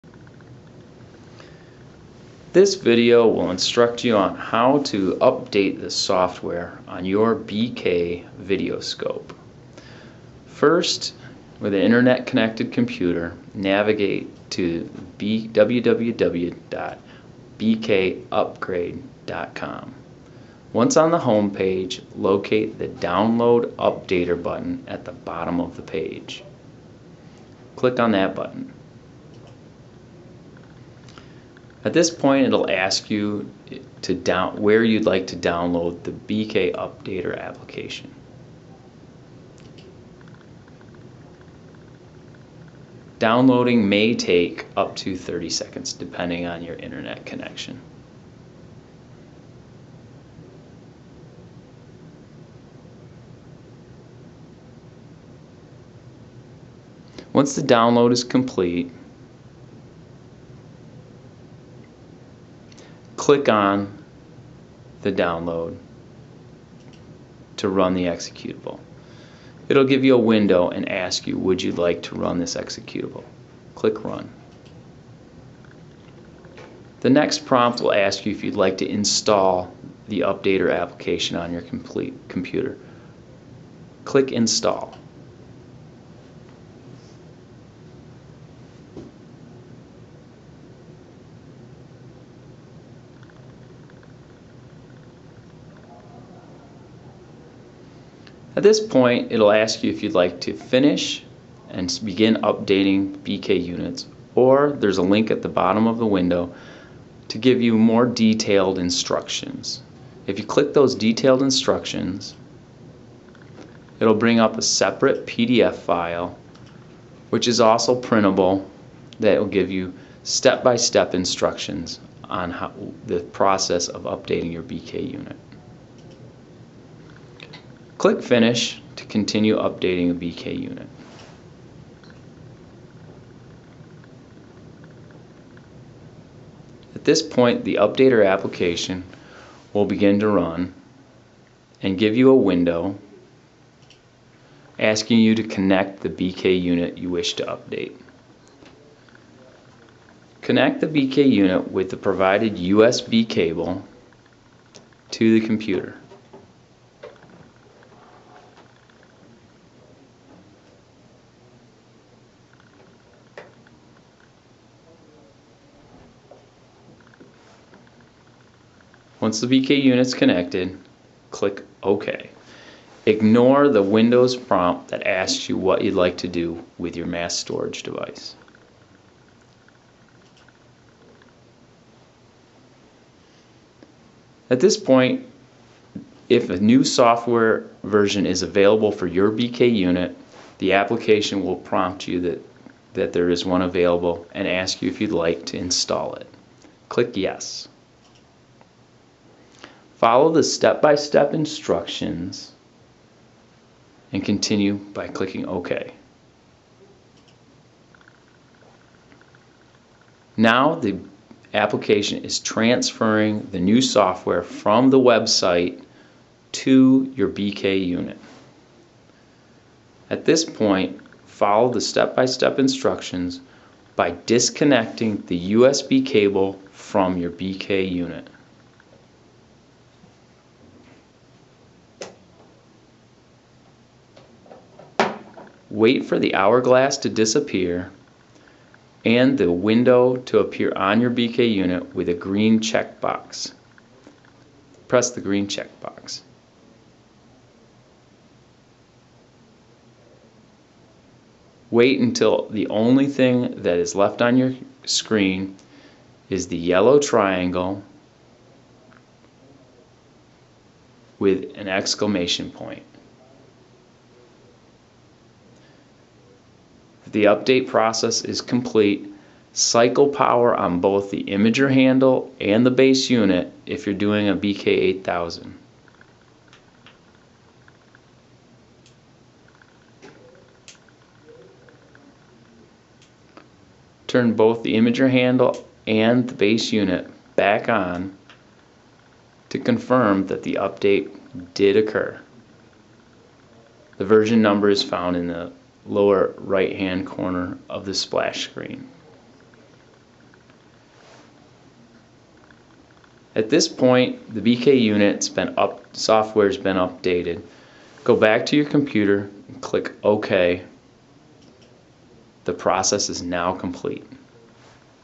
Update the software on a BK video scope
This video is a step by step instruction on how to download and update the latest software for a Snap-on BK series video scope.